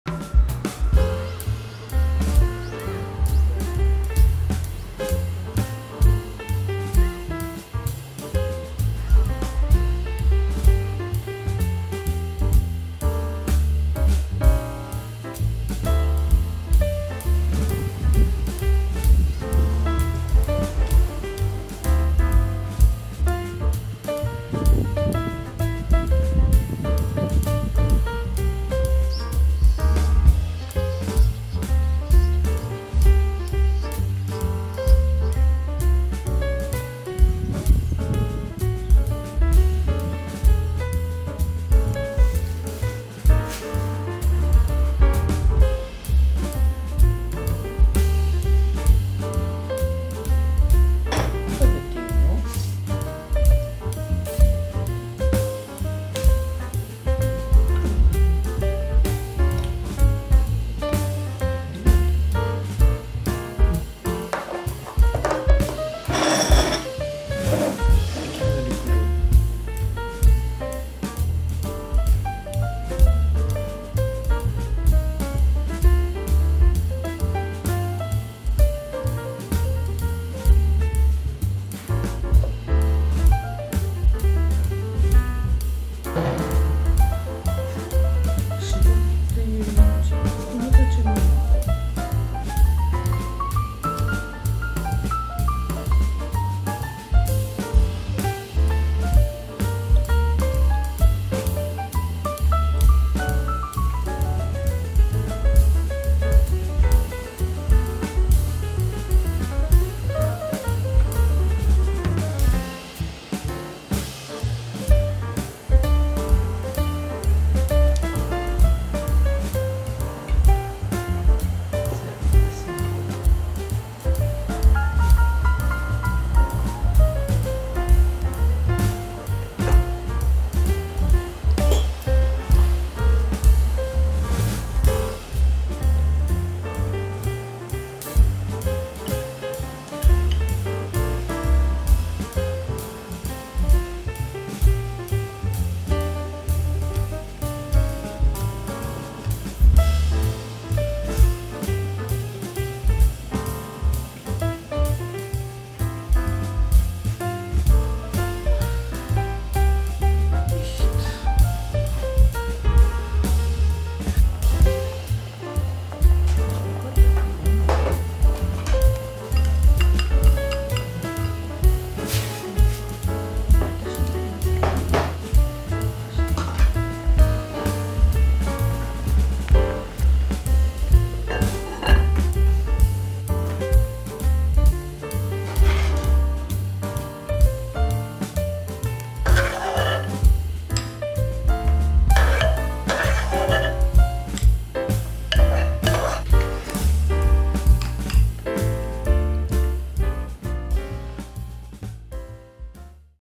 若狭ふぐ 若狭神子 「活ふぐの宿 藤乃屋」 福井の風景 2023年4月3日
今年は神子の山桜が咲くのが早かった。民宿の女将さんの話では、山肌が白く染まったのは一週間前だったとのこと。桜は残念だったけど、若狭ふぐは堪能できた。お酒は地酒「加茂栄」